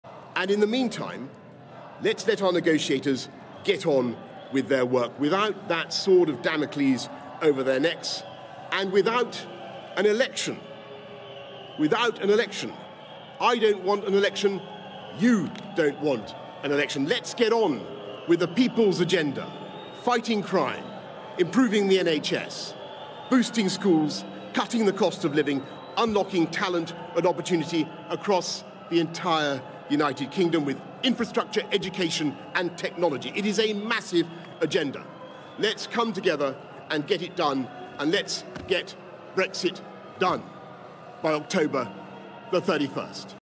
Boris Johnson insists "I don't want an election"
"I don't want an election, you don't want an election" - Prime Minister, Boris Johnson, insists that he does not want a general election.

Mr Johnson delivered the speech outside of 10 Downing Street, amid rising speculation about a general election.

and download our apps